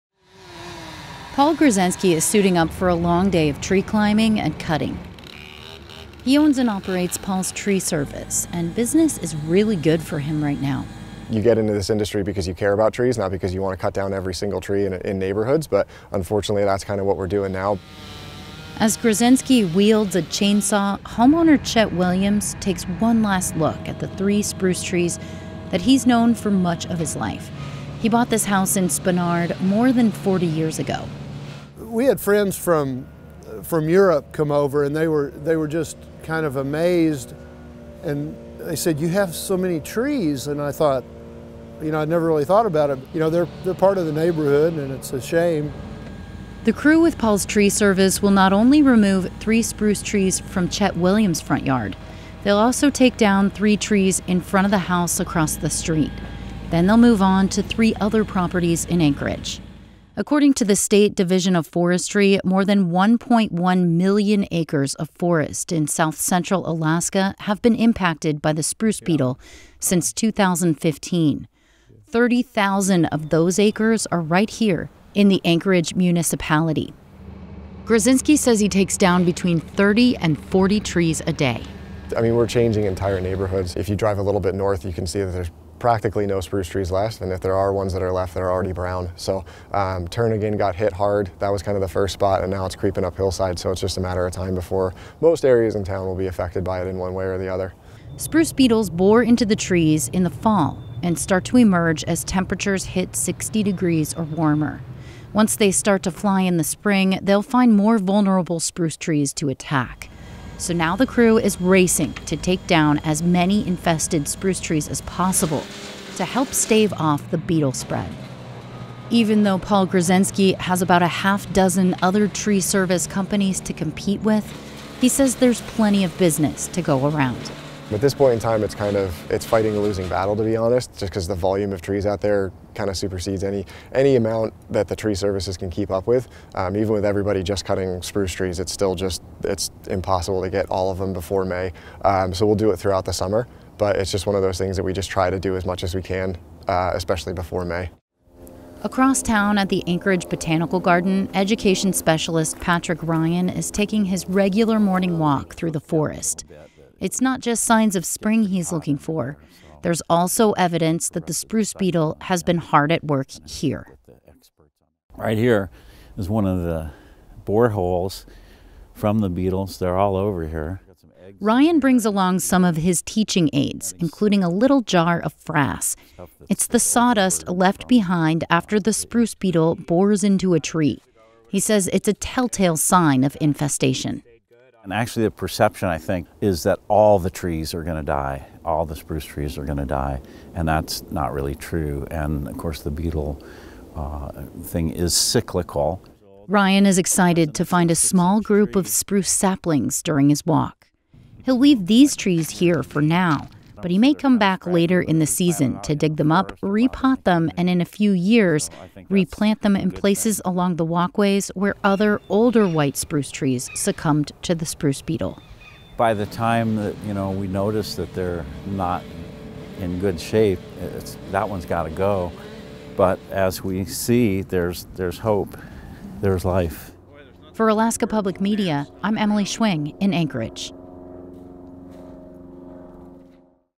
Tiny beetles threaten spruce trees and homes in Alaska
Take a flight over the Mat-Su valley, the Anchorage Bowl, or the Kenai Peninsula and you may notice areas that were once evergreen, but have now turned a rusty reddish brown. All of those trees are fuel for wildfires and a small beetle has killed many of them. As Alaska Public Media’s Emily Schwing reports, it’s not only happening in remote parts of the state. Thousands of urban trees are also coming down because of the spruce beetle.

To support content like this, consider supporting Alaska Public Media by making a financial gift